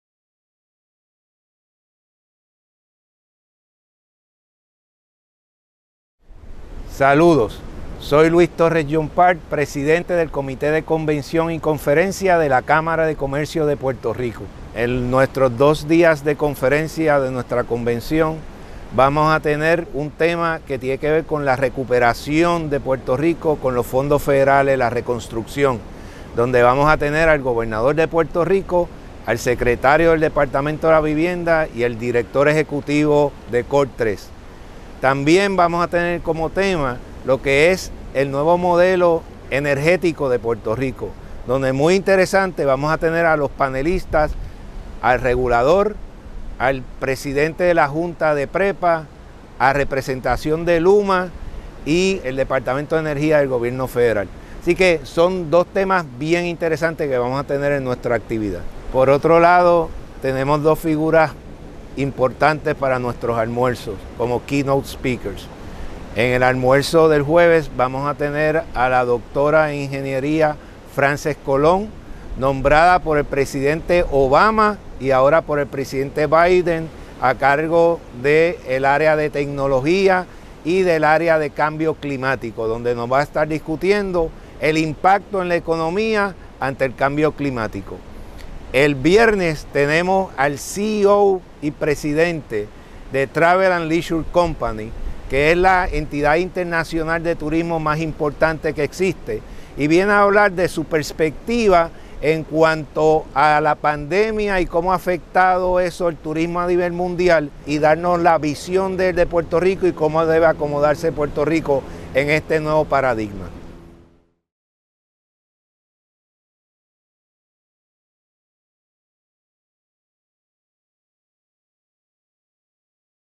Luis Torres Llompart te invita a Convención
Luis Torres Llompart, presidente del Comité de Convención BIZCON+EXPO 2022 GROW, te invita a que participes de foros y conferencias con temas relevantes y oradores de primer orden.

No te pierdas esta oportunidad única de establecer contactos, aprender y tener éxito. ¡Regístrate hoy!

PRCC BIZCON+EXPO 2022 GROW
9 y 10 de junio @CaribeHilton